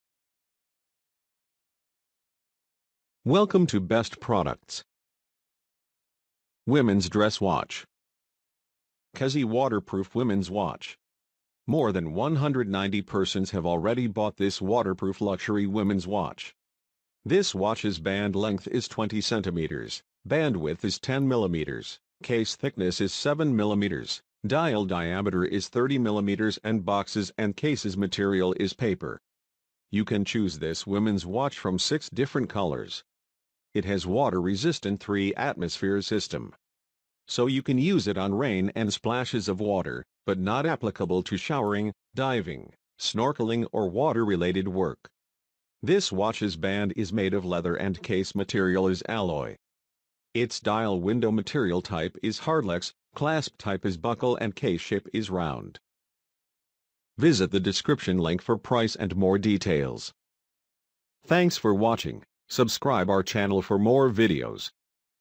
Best women's dress watch
More than 190 persons have already bought this Waterproof women's dress watch. This watches Band Length is 20cm, Band Width is 10mm, Case Thickness is 7mm, Dial Diameter is 30mm and Boxes and Cases Material is Paper. You can choose this women’s watch from 6 different colors. It has Water Resistant 3ATM system.

Overall, it can be a good choice if you are really in need of a best women's dress watch and don’t want to invest much money.